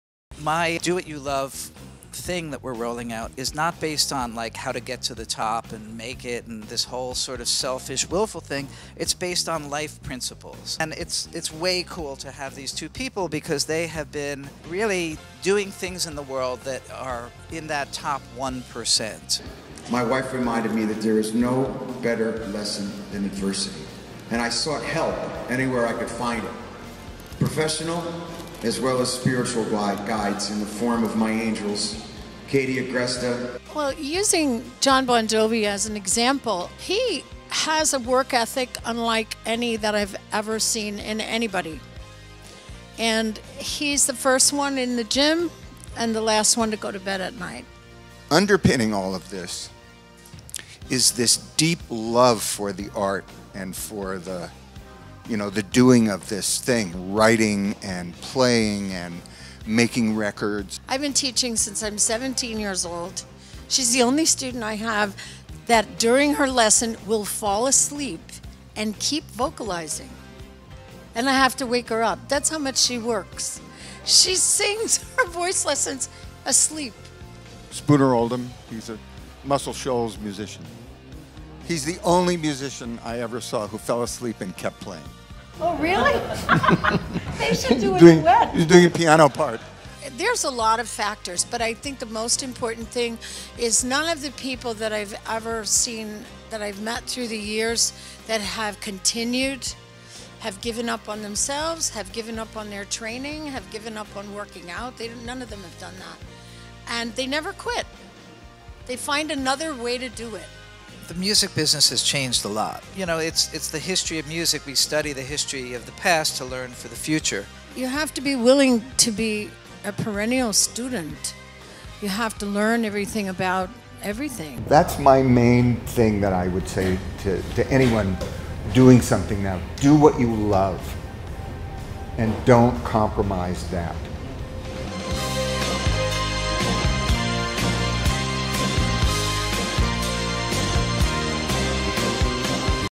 Do What You Love Media Salon with Katie Agresta & Russ Titelman
What do Jon Bon Jovi, Cyndi Lauper, Eric Clapton, Paul McCartney, Stevie Winwood, Ricki Lee Jones, George Harrison, Sting, Brian Wilson, Steven Tyler, Chaka Kahn all have in common? They have worked with one or both of these humble music legends- Katie Agresta & Russ Titelman. I sat down with both to discuss their secrets of success, stories, inspiring and funny moments on June 20, 2018. It's part of the Do What You Love Media/ Blender Workspace Entrepreneur series. It's a quick 2 minutes of gold nuggets!